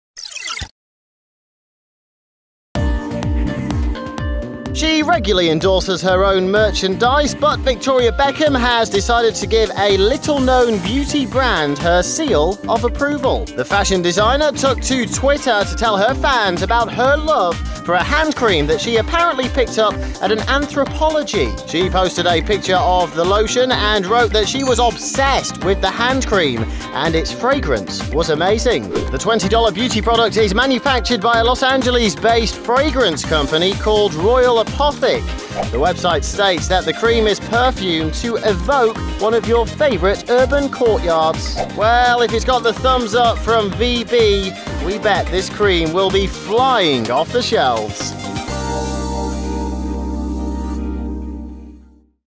Victoria Beckham Reveals $20 Hand Cream She is 'Obsessed' With | Splash News TV
Victoria Beckham reveals hand cream she is 'obsessed' with - and it costs just $20.